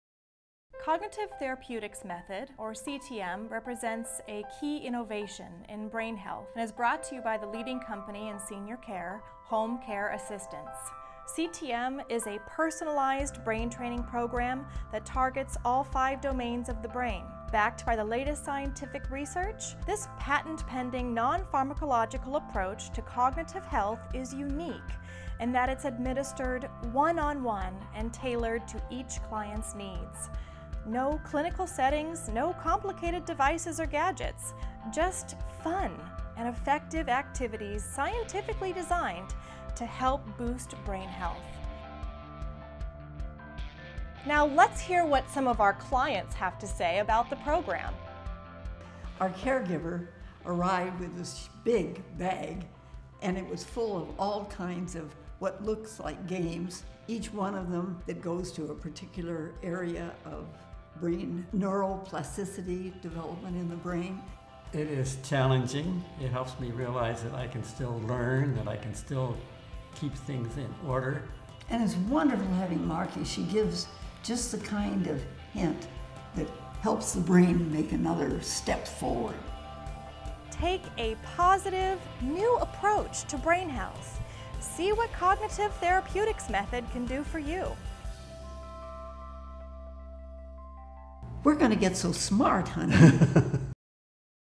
Cognitive Therapeutics Method
The Cognitive Therapeutics Method (CTM) is a patent-pending cognitive stimulation program developed by the scientific division of Home Care Assistance. The program aims to improve quality of life by enhancing mental acuity as well as to delay onset and slow the progression of symptoms of cognitive decline.